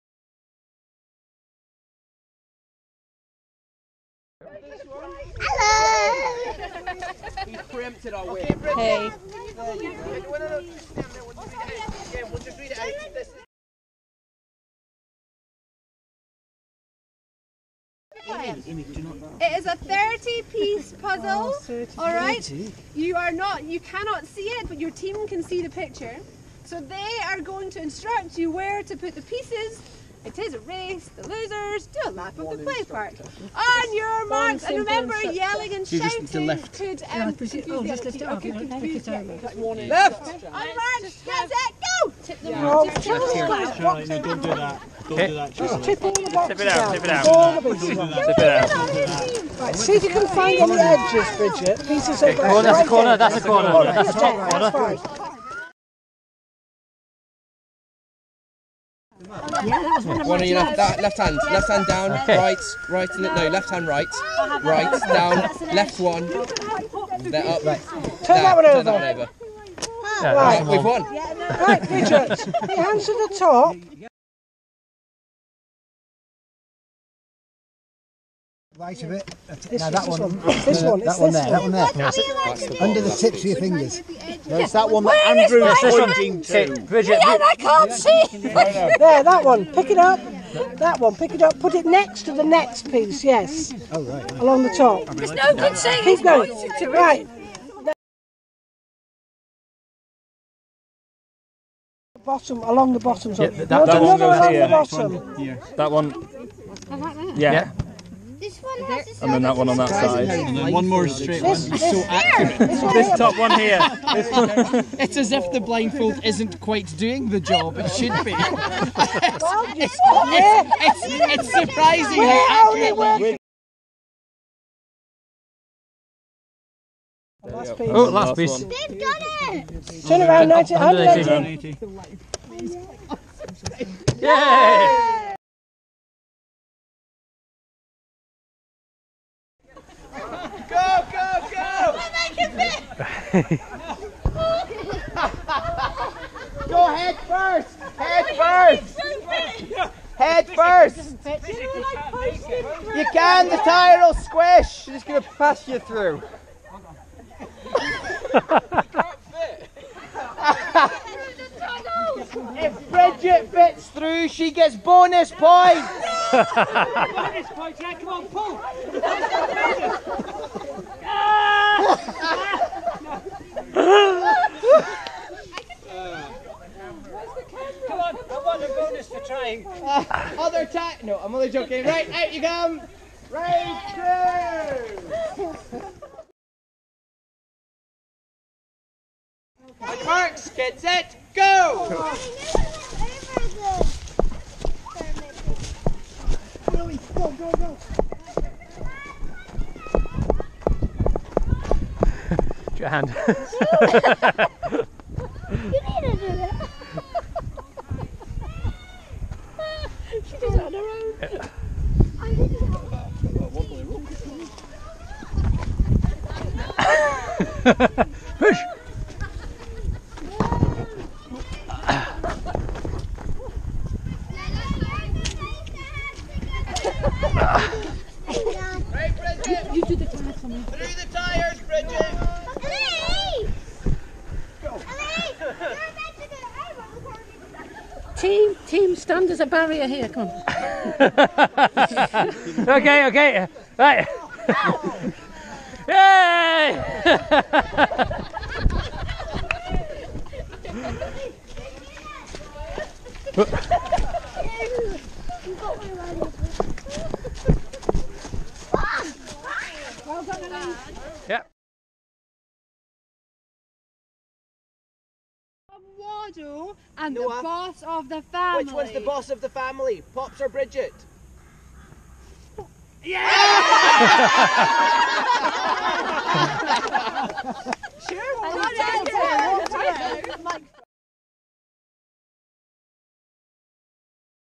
Waddow 2017 outdoor games
A few snippets from the outdoor games. Charlie's team vs Bridget's.